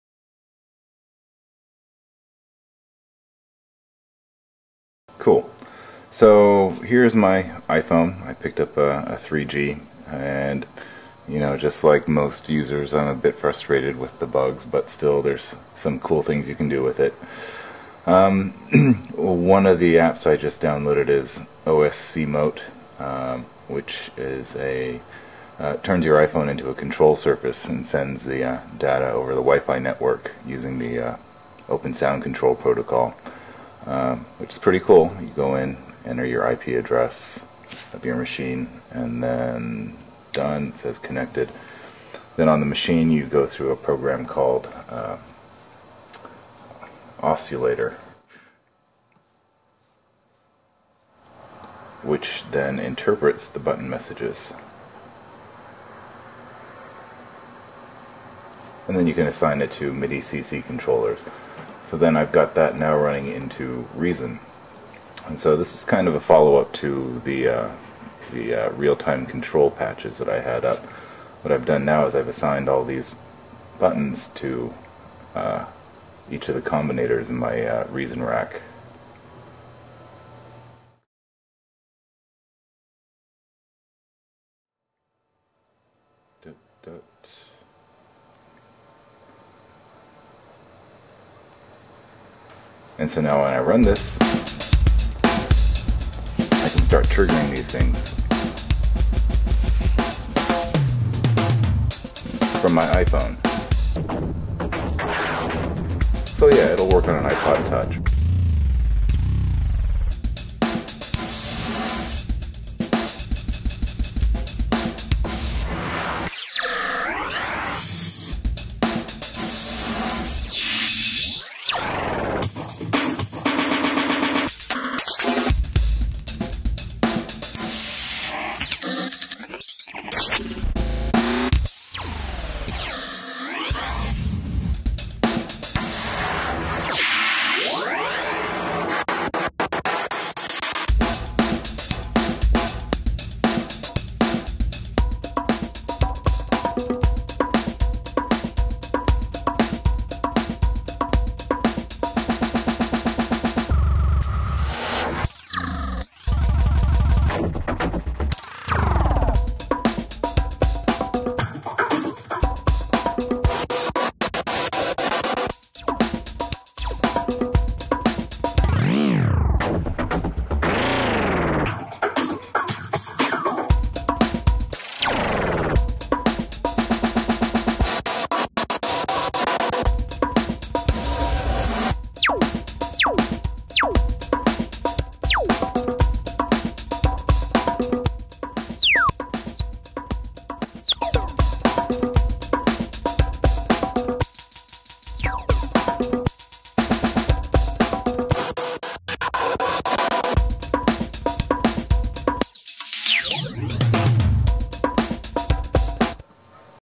iPhone Control - Reason 4.0 Combinators
Using the OSC tools for a non-jailbroken iphone to trigger combinator effects in Reason 4.0.   This relies on OSCulator, the application popularly used for the Wiimote interface, and the OSCemote iPhone application.